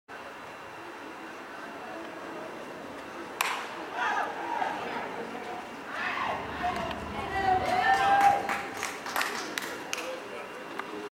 2ND BASE Fly CATCH for OUT by 16U Chayton Beck - 2018 Sophomore PG Series Classic, Ft Myers FL
2nd Base Fly Catch makes Out by 16u Chayton Beck - 2018 PG Sophomore Series Classic, Ft Myers Fl